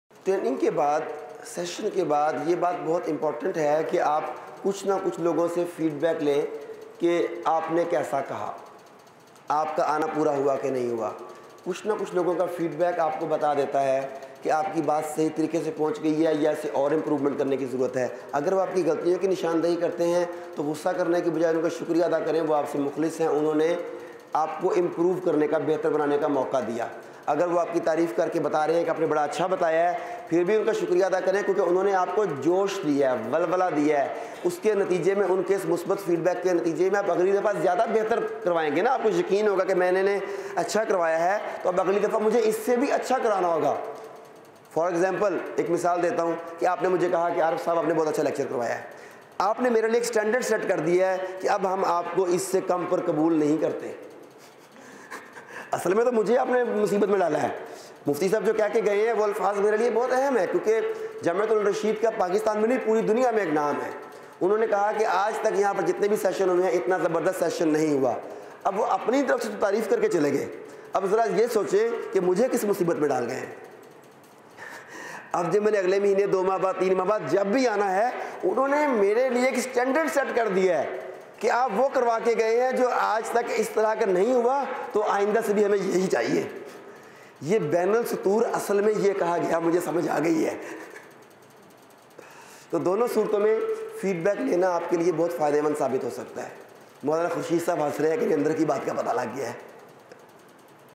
Feedback is helpful || Dr. Arif Siddiqui || Public Speaking Trainer || Jamia Tur Rasheed
Feedback is a tool that can assist people in evaluating themselves, their work, and how others view them.  The opinions of others reveal information about the quality of your job. Or you need to put forth more effort.  If they point out your mistakes, thank them for giving you the chance to fix them. On the other hand, if they compliment you on your performance, thank them again because they are pushing you to do better. Feedback is helpful || Dr. Arif Siddiqui || Public Speaking Trainer || Jamia Tur Rasheed